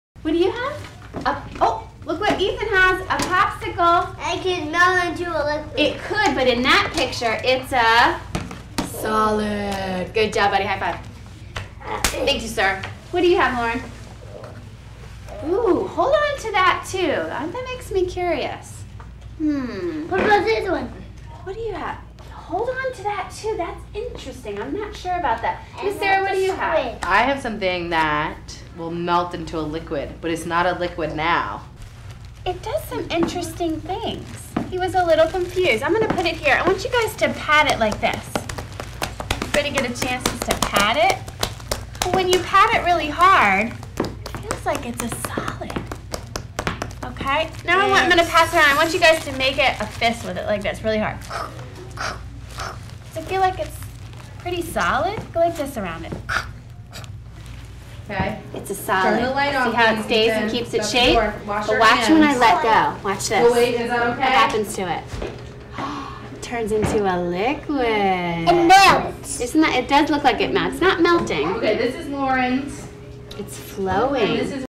Curiosity Zone Kids Learn About Liquids & Solids
Preschool kids put on little lab coats and dig into oobleck to learn about liquids and solids!  Curiosity Zone is a hands-on science center for curious kids ages 1-11.